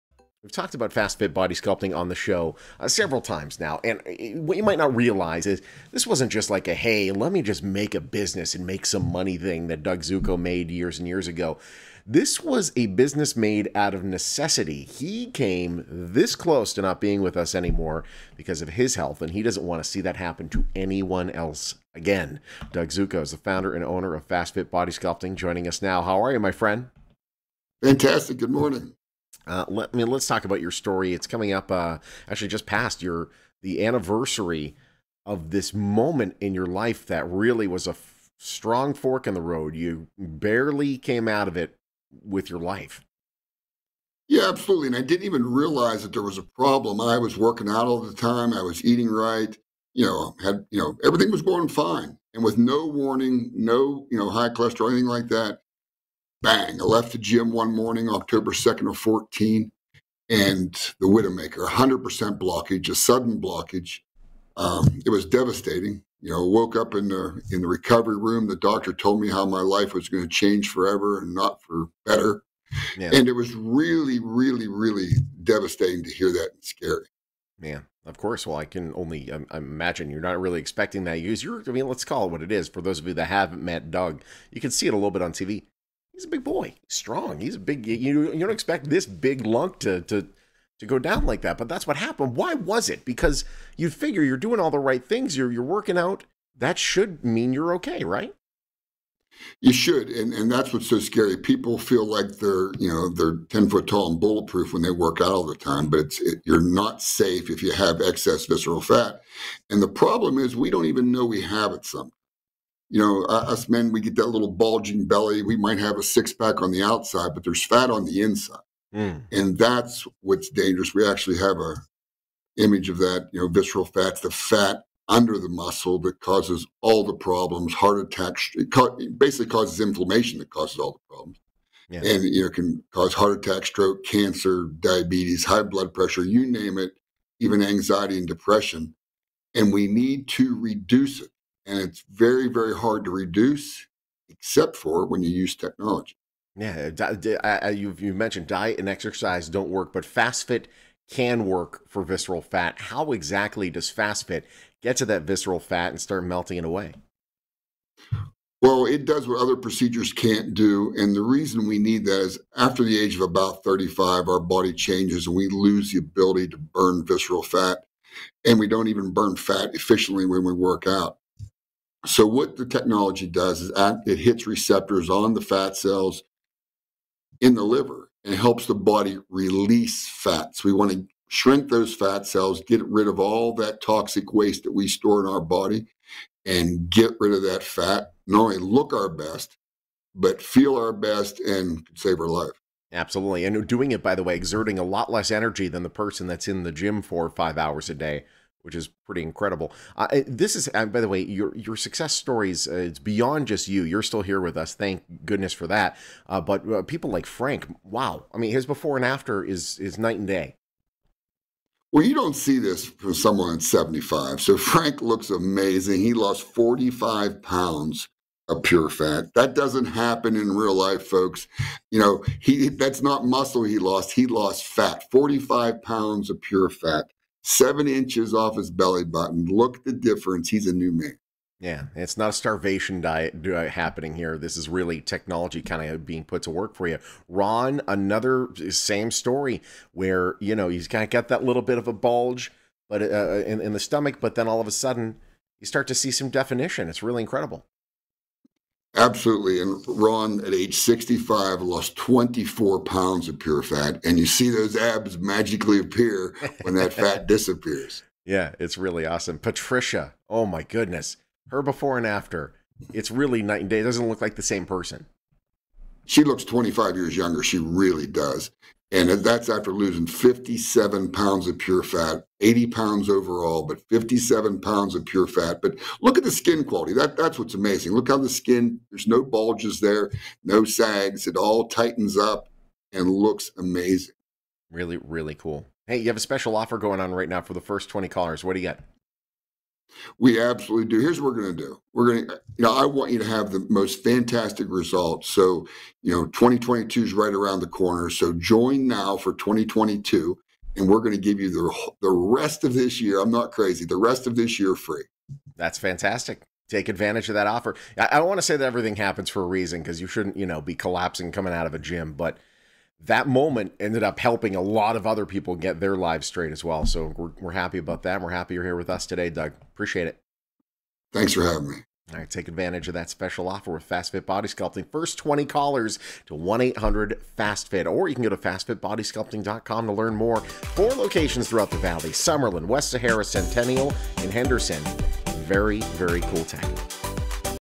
Fast Fit: Doug's Story
Hear Doug's transformation story and how Fast Fit Body Sculpting came to be!